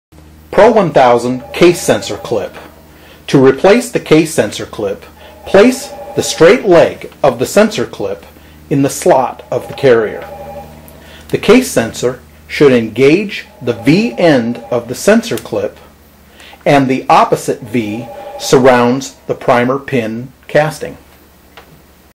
How To Replace the Case Sensor Clip on a Lee Precision Pro 1000
In this video, FSReLoading will show you how to properly replace the case sensor clip on a Lee Precision Pro 1000 ammunition re-loader.